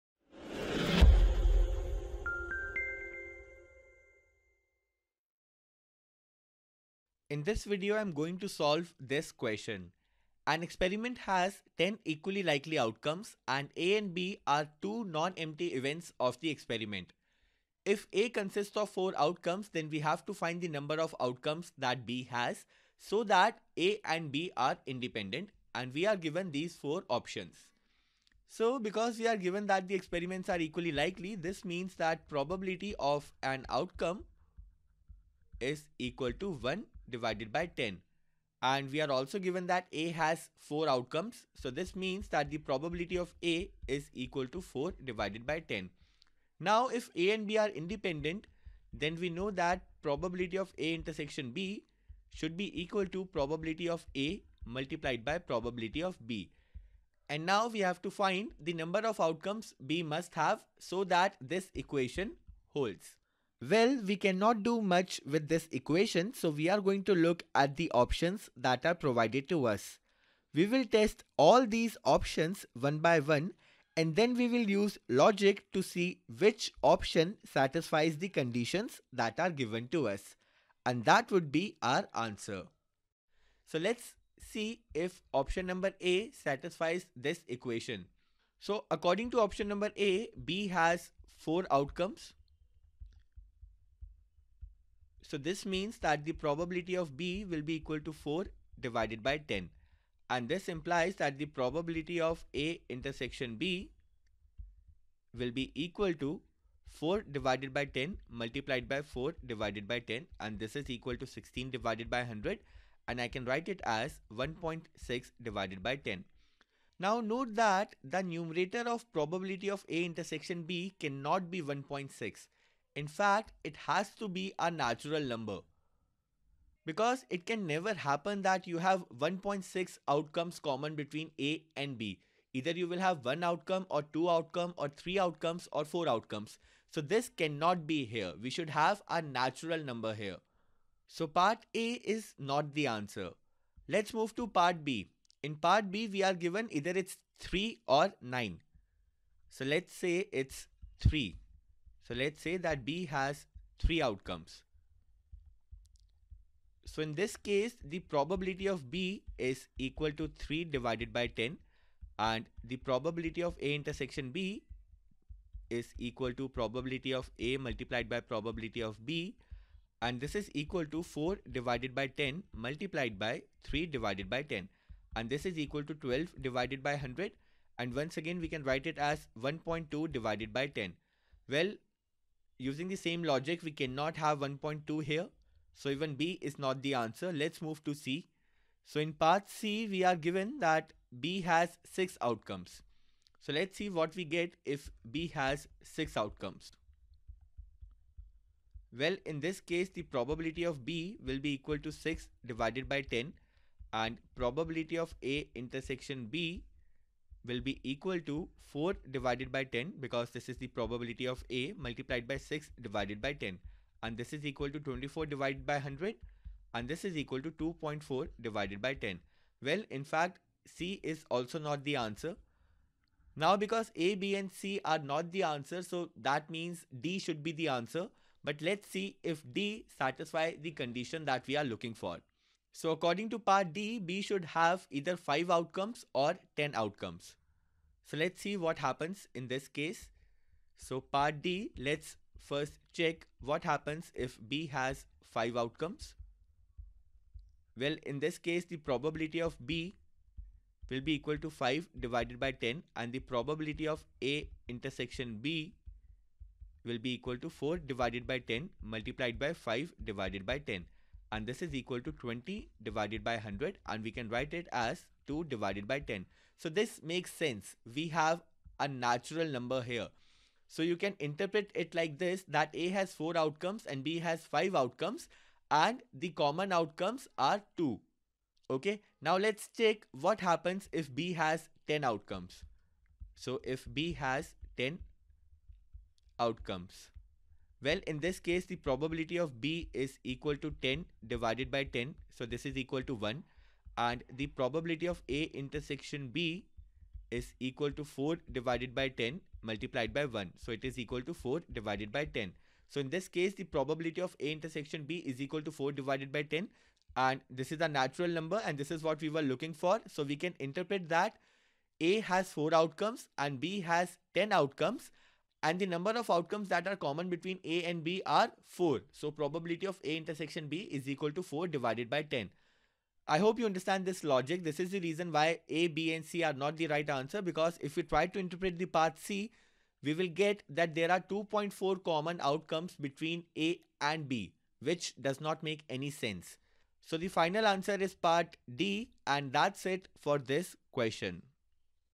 DSE 2012 Q43 (M.A. Economics Entrance Exam)
Join our Broadcast list for M.A. Economics Entrance Exam and stay updated on the video content.

Question : An experiment has 10 equally likely outcomes. Let A and
B be two non-empty events of the experiment. If A consists of 4 outcomes,
then the number of outcomes B must have so that A and B are independent,
is
(a) 4
(b) 3 or 9
(d) 5 or 10

0:00 Eduspred
0:07 Introduction to DSE 2012 - Question 43
0:29 Solution